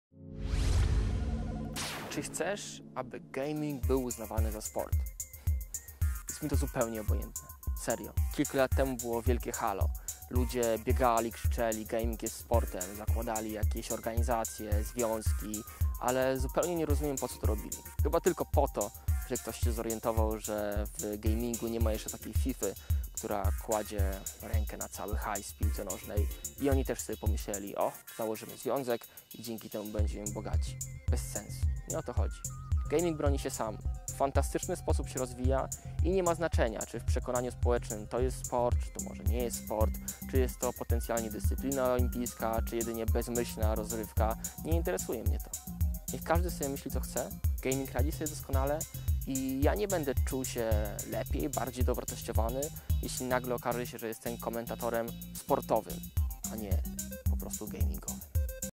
Czy gaming jest sportem? | SawikTV
W tej serii odpowiadam na zadane przez Was pytania. Pytania możecie pisać w komentarzach na YT lub na FB